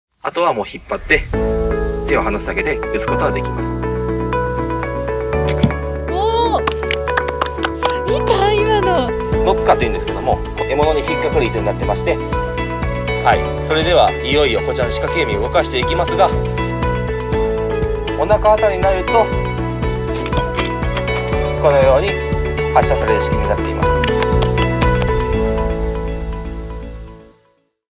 仕掛け弓 実演・解説
狩猟で用いた弓や罠などの道具の仕組み・使用する場所、捕獲動物に合わせた使用方法について、解説とともに実演します。
※イメージ映像　実際の体験内容と異なる場合があります